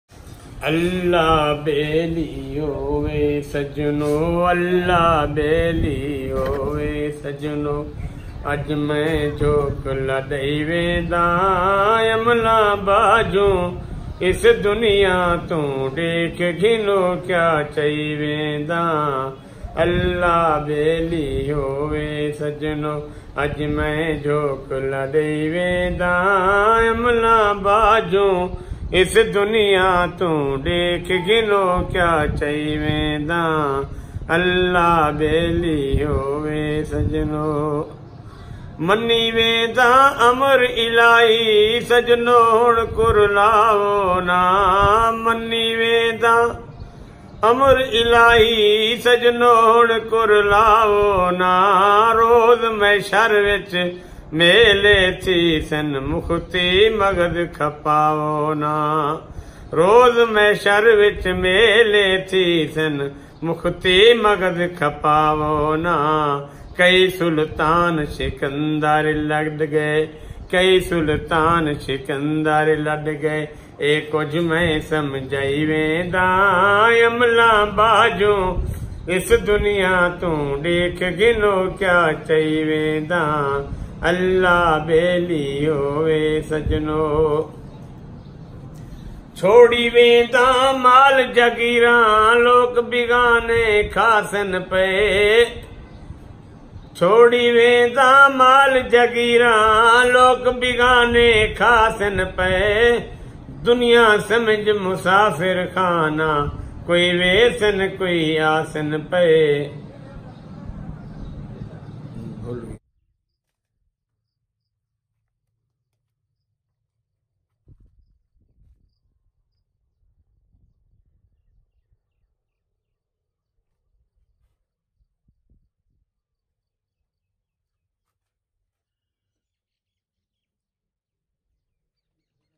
Beautiful heart touching naat sharif
Beautiful voice Muhmmad bakish naat sharif with beautiful natural view